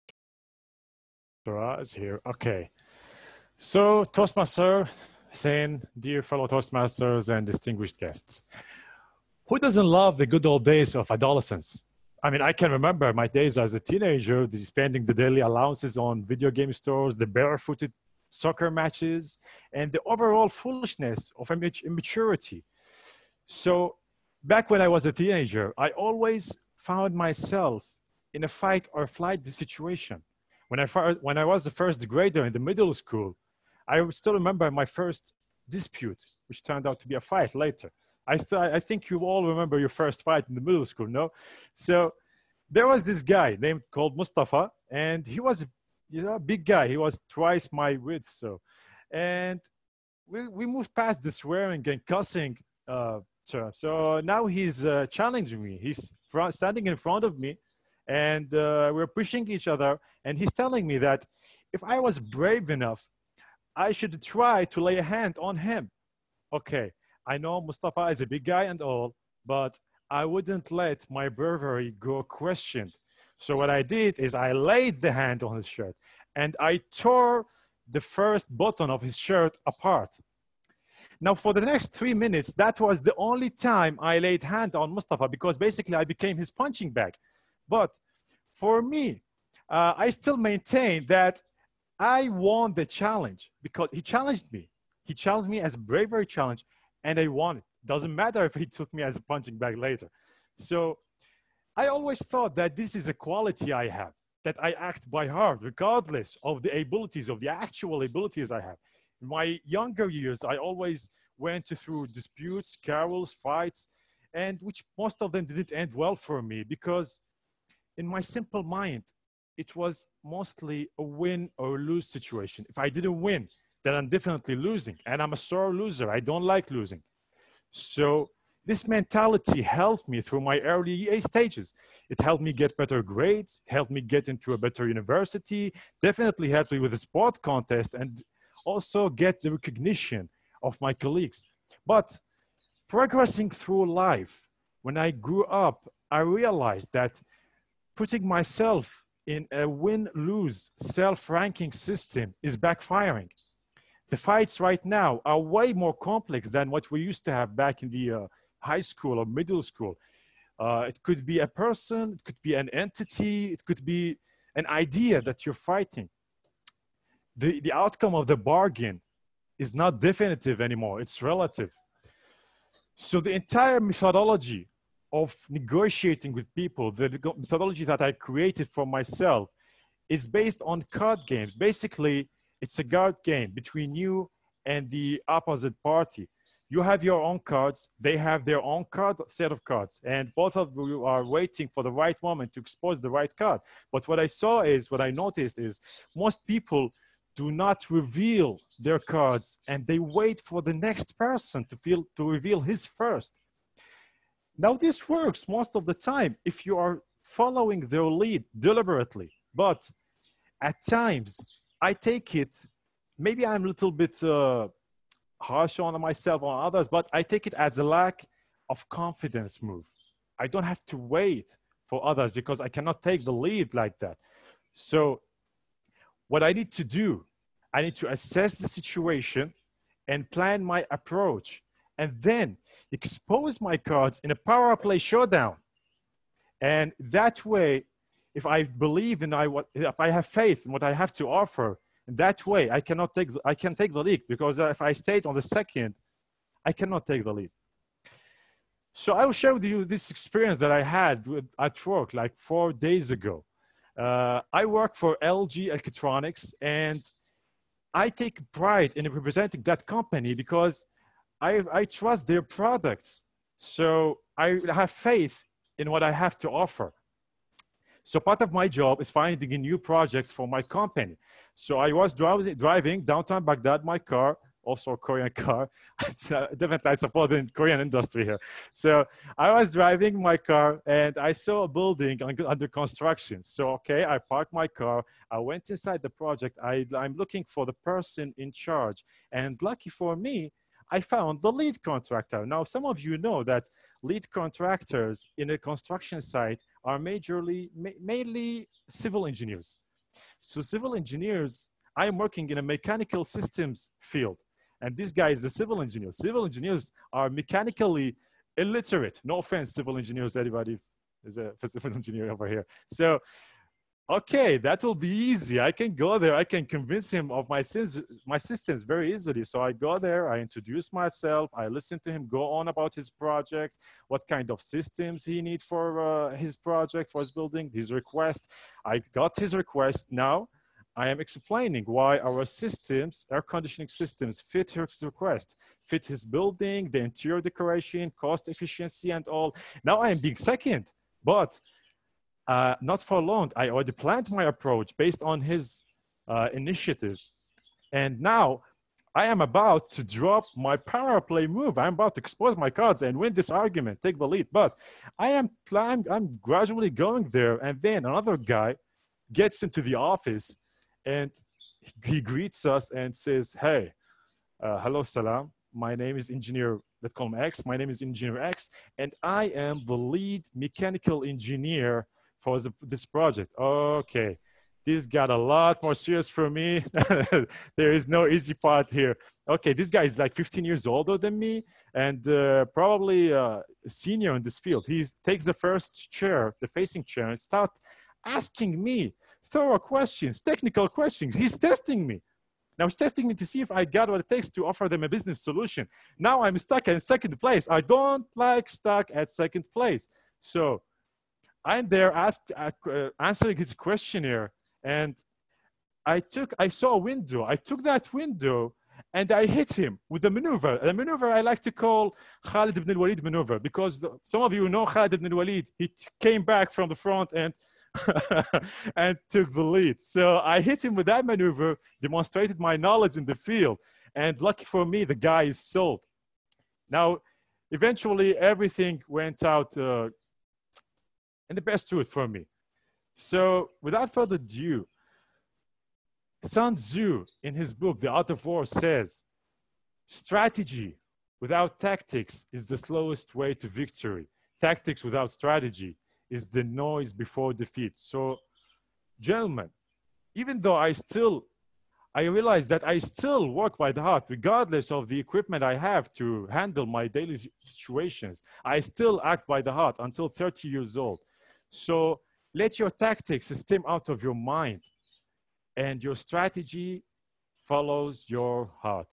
Negotiate the best outcome; Salam Al-Saffar
The president of Baghdad Toastmasters delivered a well-organized speech about negotiation. The speech contains valuable information about getting the best outcome while negotiating, he enriched the speech with his personal experience regarding this aspect.  DON'T Miss it!!